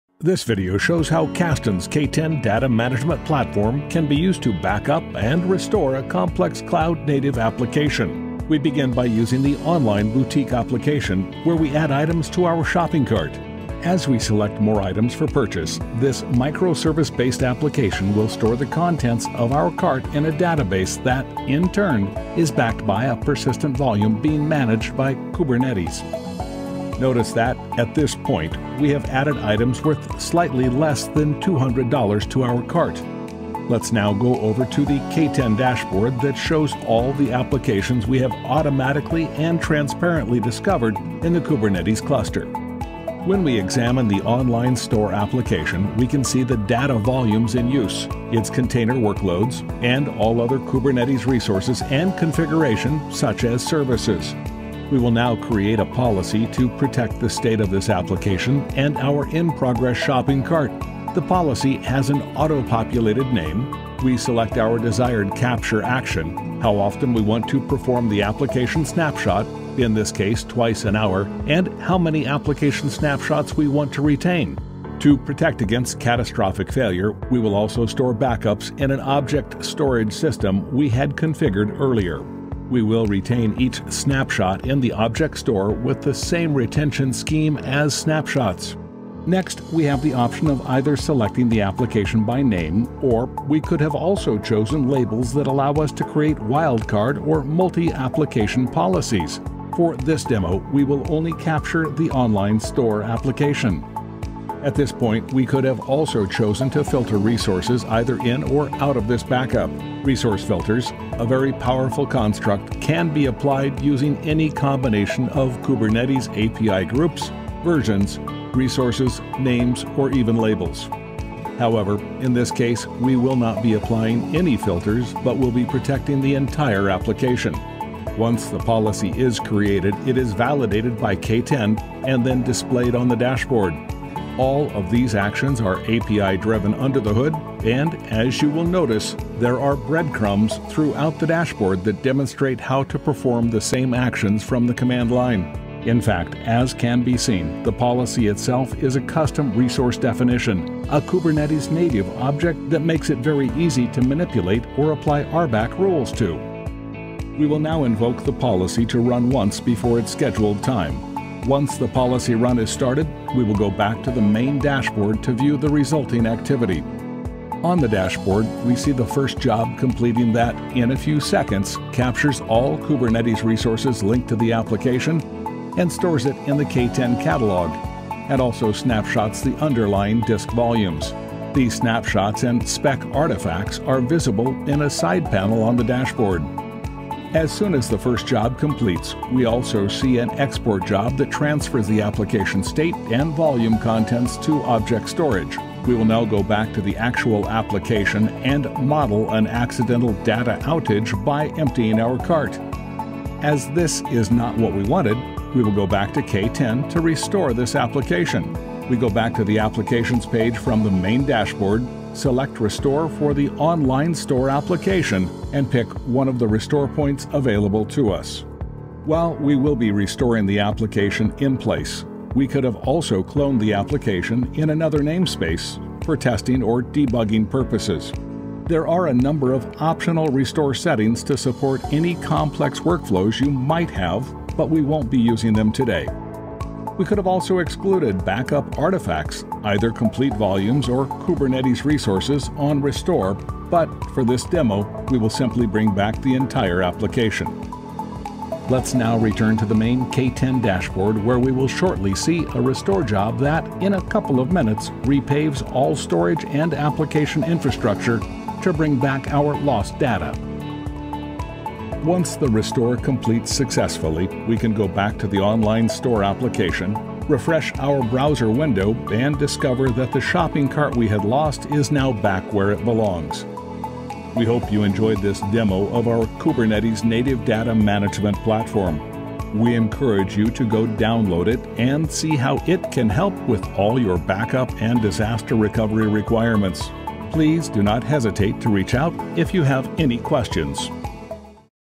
Kubernetes Backup and Restore with Kasten K10 Data Management Platform
This video shows how Kasten's K10 can transparently protect an off-the-shelf Kubernetes-native microservice-based application and demonstrates how backup and recovery is performed without requiring any application changes.